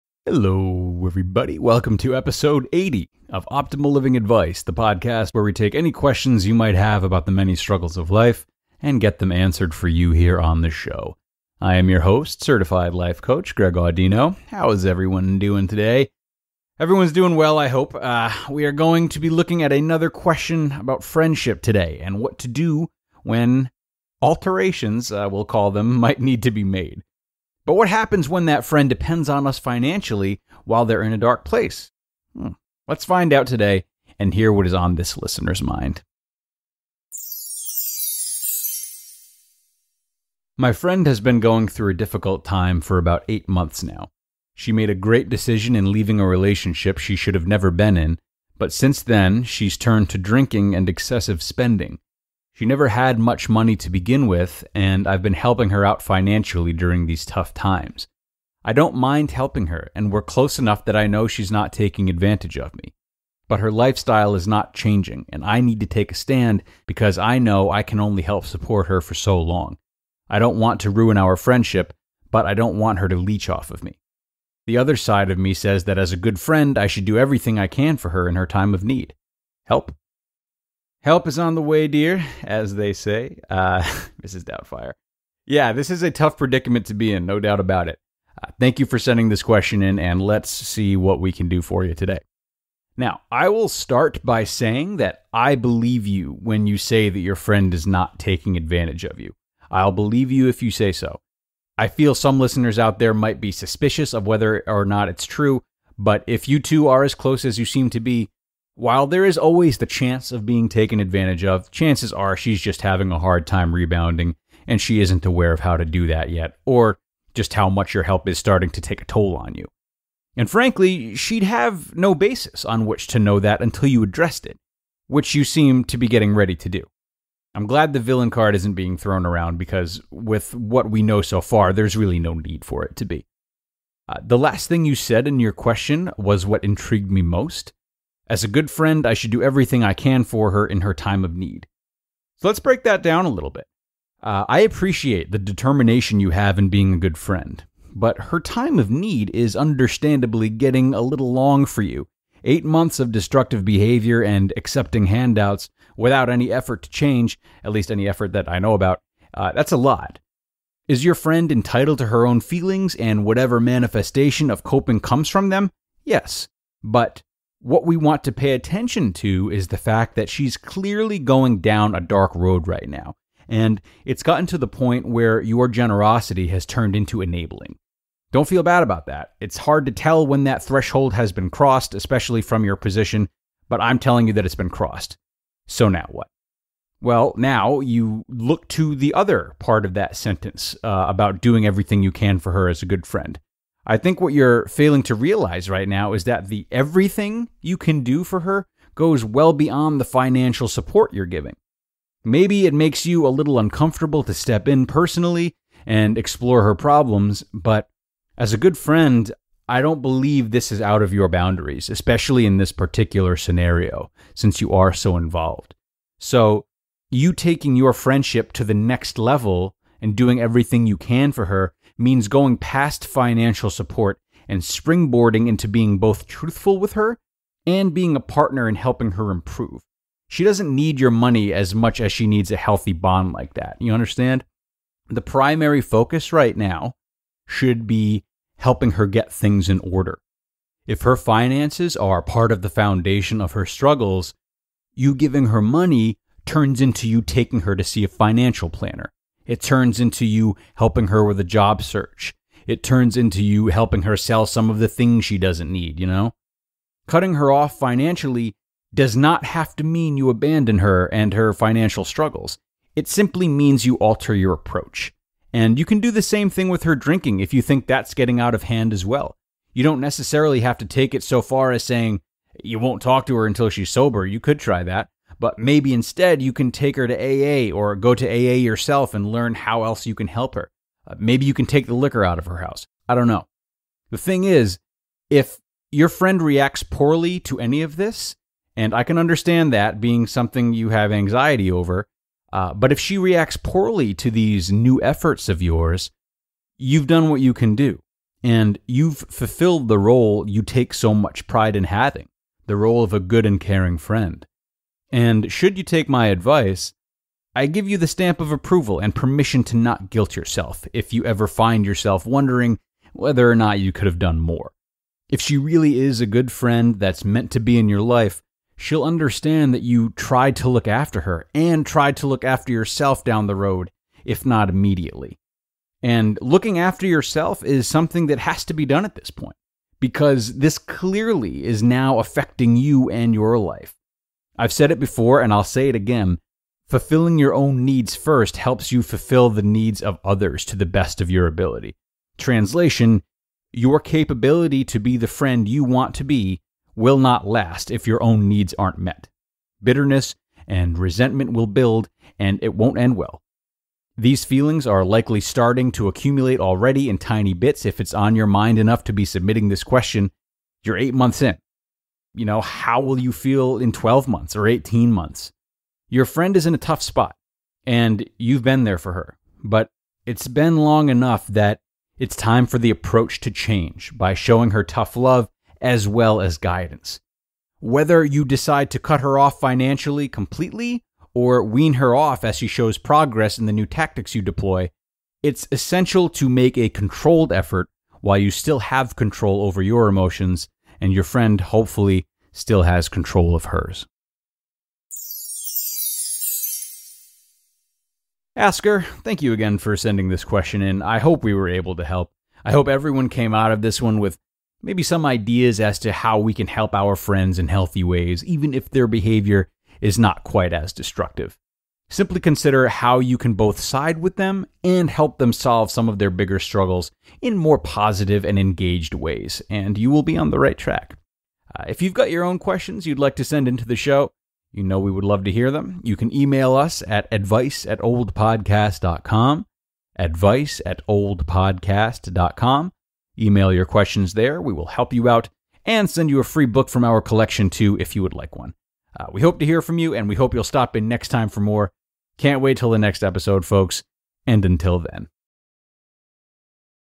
080: Steering Friends in the Right Direction - How to Give to a Friend in Need & When Generosity...
Greg Audino shares his thoughts on steering friends in the right direction.

 Episode 080: Steering Friends in the Right Direction - How to Give to a Friend in Need &amp; When Generosity Becomes Enabling